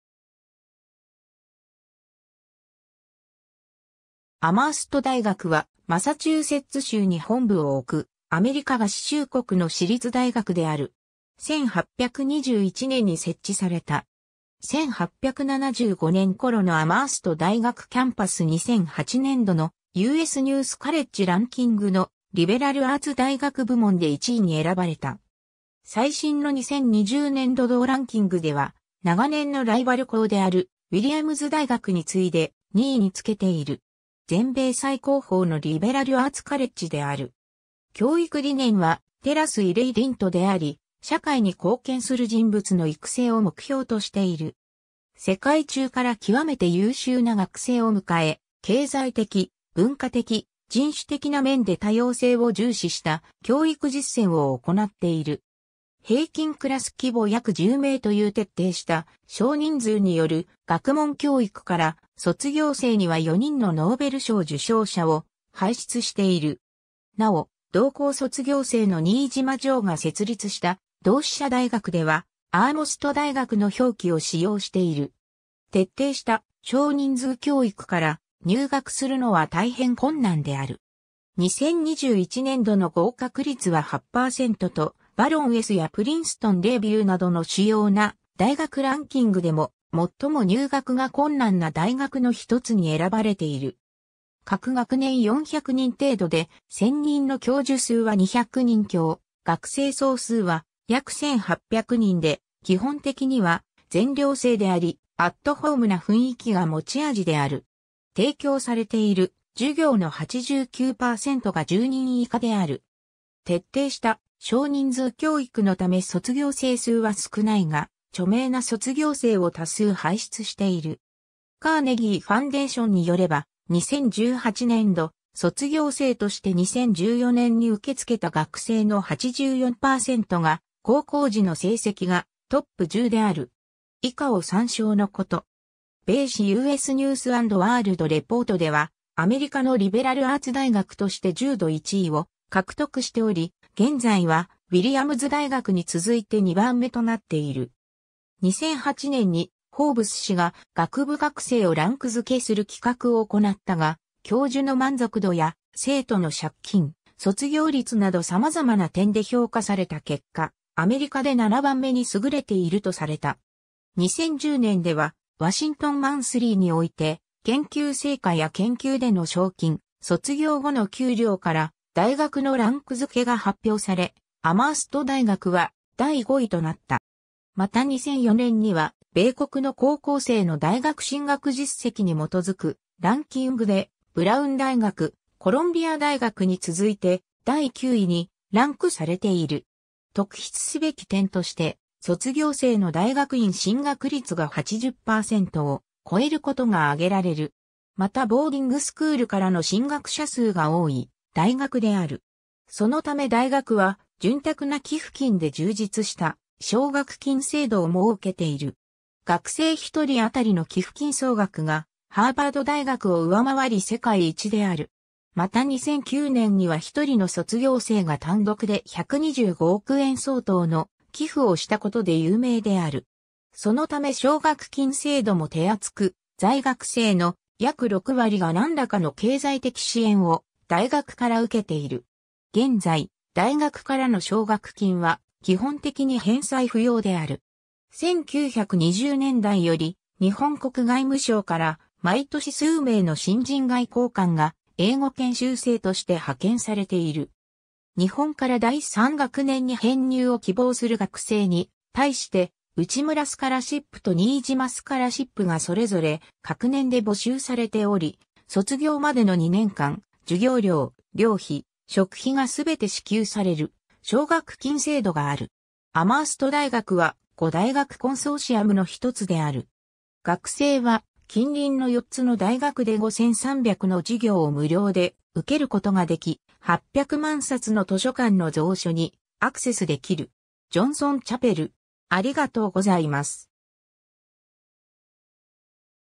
アマースト大学
アマースト_(マサチューセッツ州)
リベラル・アーツ・カレッジ
アマースト大学（英語: Amherst College）は、マサチューセッツ州に本部を置くアメリカ合衆国の私立大学である。
1821年に設置された。
 1875年ころのアマースト大学キャンパス 2008年度のU.S News College Rankingのリベラルアーツ大学部門で1位に選ばれた。
最新の2020年度同ランキングでは、長年のライバル校であるウィリアムズ大学についで、2位につけている。
全米最高峰のリベラルアーツ・カレッジである。
教育理念は"Terras Irradient (Let them give light to the world)"であり、社会に貢献する人物の育成を目標としている。
世界中から極めて優秀な学生を迎え、経済的・文化的・人種的な面で多様性を重視した教育実践を行っている。
平均クラス規模約10名という徹底した少人数による学問教育から、卒業生には4人のノーベル賞受賞者を輩出している。
 なお、同校卒業生の新島襄が設立した同志社大学では「アーモスト大学」の表記を使用している。
 徹底した少人数教育から、入学するのは大変困難である。
2021年度の合格率は8%と、Barron'sやPrinceton Reviewなどの主要な大学ランキングでも「最も入学が困難な大学（Most Competitive）」のひとつに選ばれている。
各学年400人程度で専任の教授数は200人強、学生総数は約1800人で、基本的には全寮制であり、アットホームな雰囲気が持ち味である。
提供されている授業の89%が10人以下である。
徹底した少人数教育のため卒業生数は少ないが、著名な卒業生を多数輩出している。
Carnegie Foundation　によれば、2018年度卒業生として2014年に受け付けた学生の84%が高校時の成績がトップ10である。
以下を参照のこと。
 米誌USニュース・アンド・ワールド・レポートでは、アメリカのリベラルアーツ大学として10度1位を獲得しており、現在はウィリアムズ大学に続いて2番目となっている。
 2008年にフォーブス誌が学部学生をランク付けする企画を行ったが、教授の満足度や生徒の借金、卒業率など様々な点で評価された結果、アメリカで7番目に優れているとされた。
2010年では、ワシントン・マンスリーにおいて研究成果や研究での賞金、卒業後の給料から大学のランク付けが発表され、アマースト大学は第5位となった。
また2004年には米国の高校生の大学進学実績に基づくランキングで、ブラウン大学、コロンビア大学に続いて第9位にランクされている。
 特筆すべき点として、卒業生の大学院進学率が80%を超えることが挙げられる。
またボーディングスクール（寄宿制私立高等学校）からの進学者数が多い大学である。
そのため大学は潤沢な寄付金で充実した奨学金制度を設けている。
学生一人当たりの寄付金総額がハーバード大学を上回り世界一である。
また2009年には一人の卒業生が単独で125億円相当の寄付をしたことで有名である。
そのため奨学金制度も手厚く、在学生の約6割が何らかの経済的支援（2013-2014年度で一人当たりの経済的支援額は平均46,809ドルに及ぶ）を大学から受けている。
現在、大学からの奨学金は基本的に返済不要である。
1920年代より日本国外務省から、毎年数名の新人外交官が英語研修生として派遣されている。
日本から第3学年に編入を希望する学生に対して、内村スカラシップと新島スカラシップがそれぞれ隔年で募集されており、卒業までの2年間、授業料・寮費・食費がすべて支給される奨学金制度がある。
 アマースト大学は「五大学コンソーシアム」の一つである。
学生は、近隣の4つの大学で5300の授業を無料で受けることができ、800万冊の図書館の蔵書にアクセスできる。
 ジョンソン・チャペル（チャペル内にはクーリッジ米大統領と新島襄の肖像画が掲げられている）